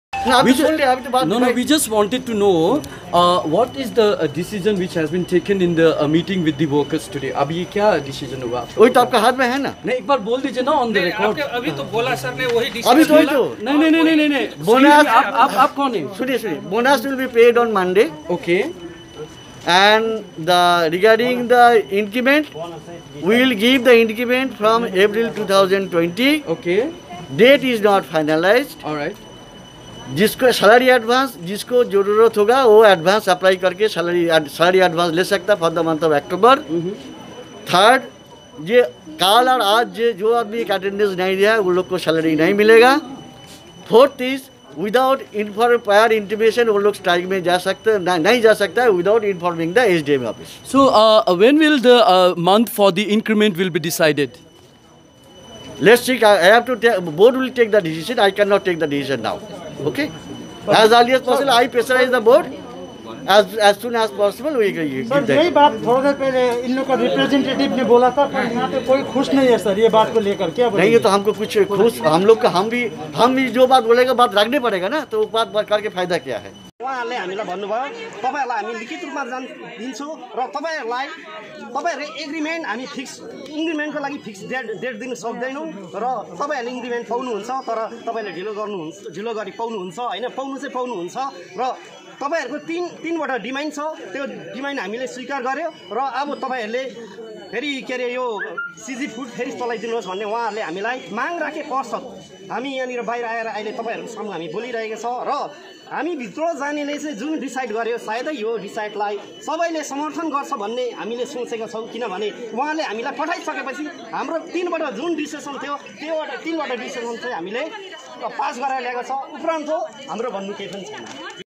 Workers of Wai Wai factory (Rangpo) staged sit in protest demanding increment.
Workers of Wai Wai  factory (Rangpo) staged sit in protest demanding increment. Find more in video below-